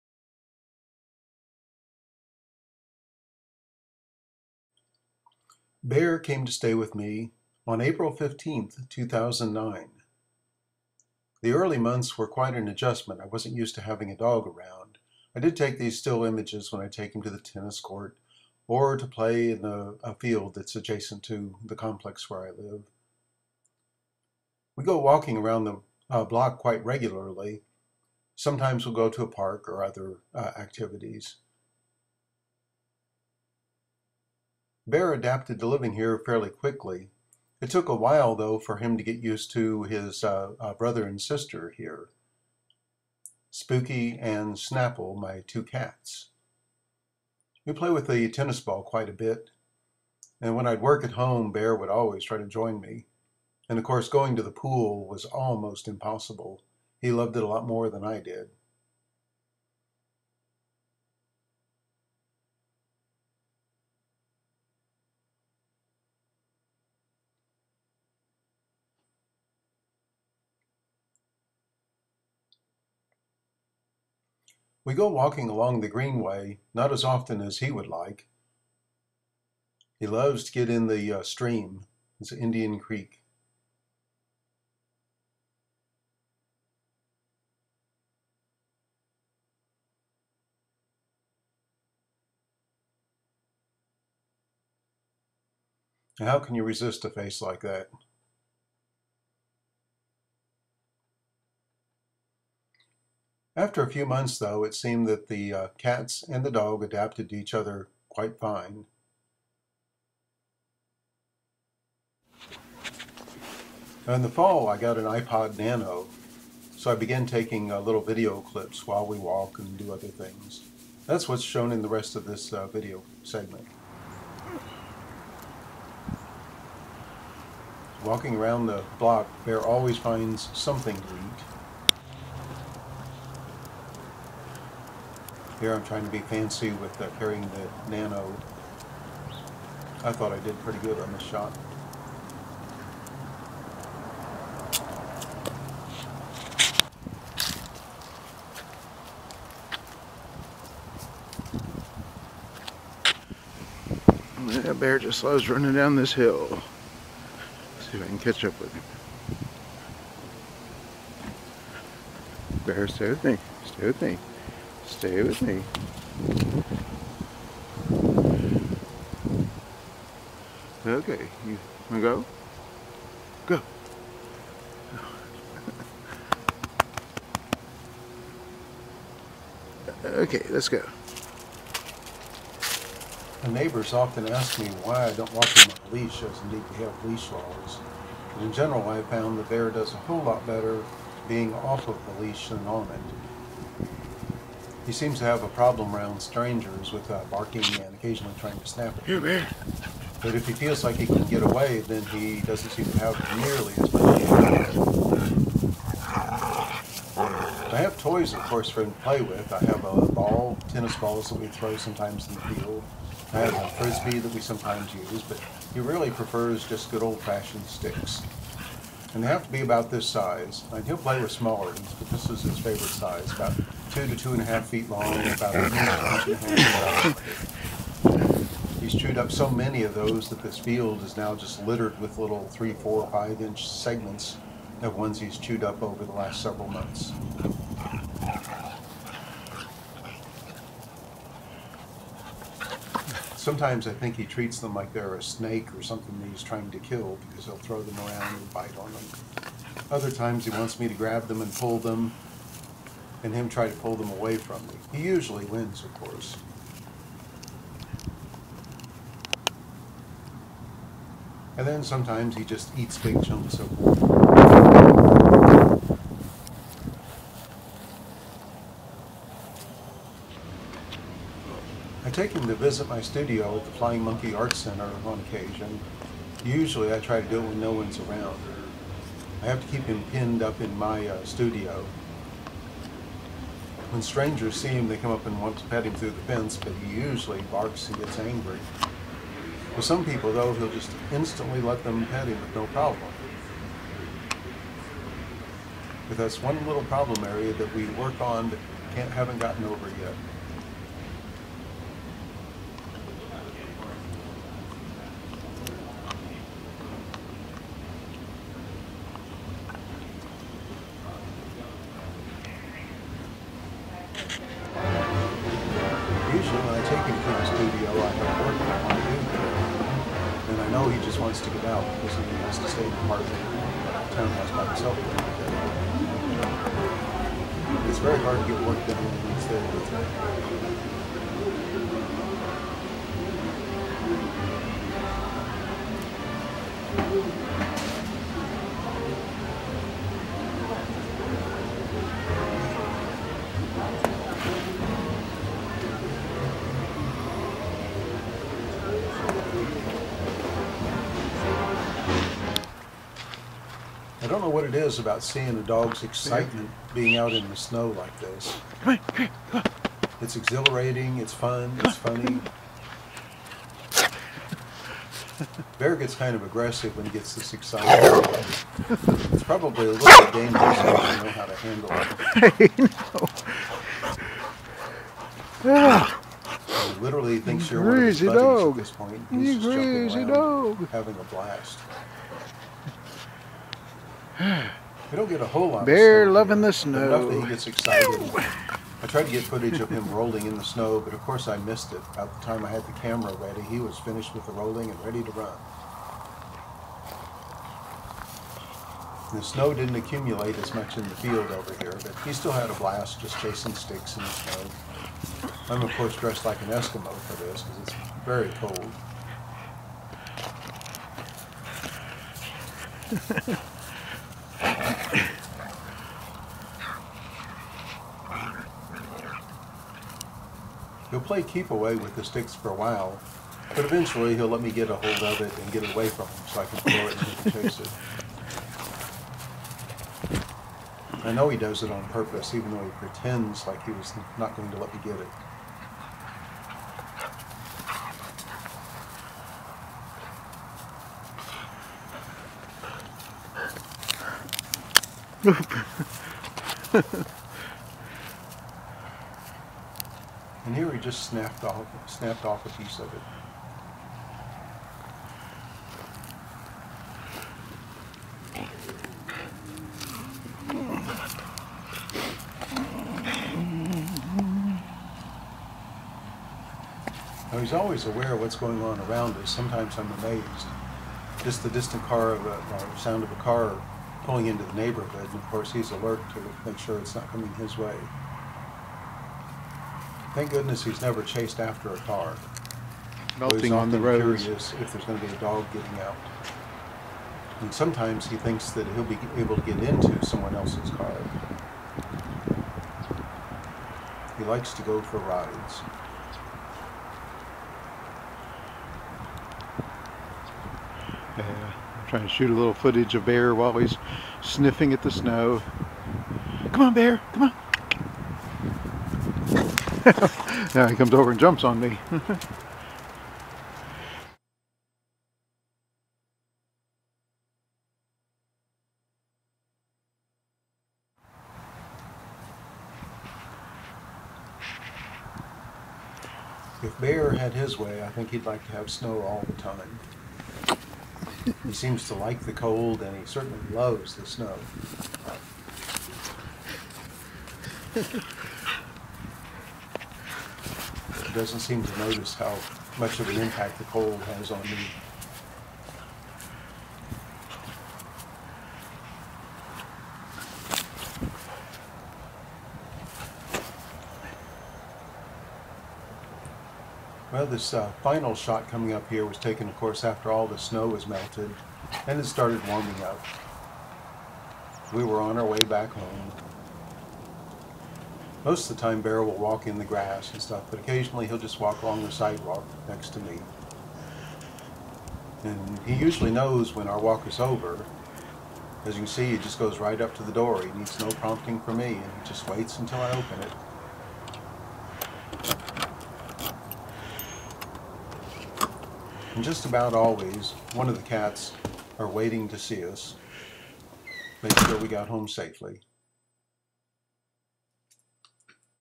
Bear - it's a dog's life
Bear moved in with me on April 15, 2009.  Here are some photos and video clips of his first year with me.  He's a mixed breed, highly intelligent, very affectionate, but leery of strangers. Gets along well with cats, other dogs, a few people and goes nutsoid with squirrels.  The video clips here were all shot with an iPod Nano.